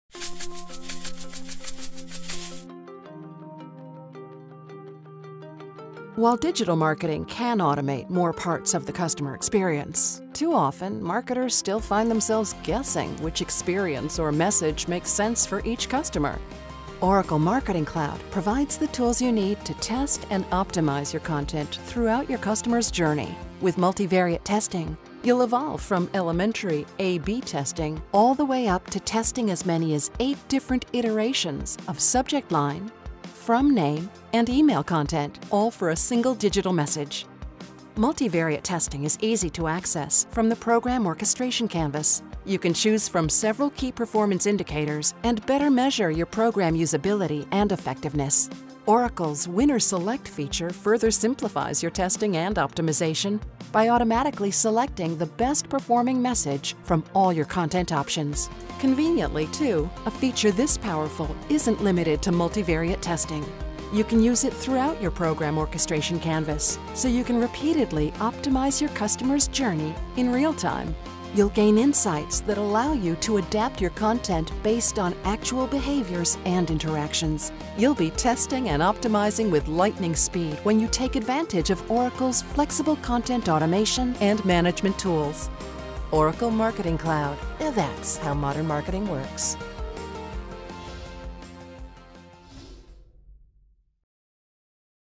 Oracle Responsys Multivariate Testing
Oracle Marketing Cloud offers the most powerful solution for optimizing the online customer experience with testing, insights, and personalization by analyzing campaigns, driving conversion rates, and generating customer insights.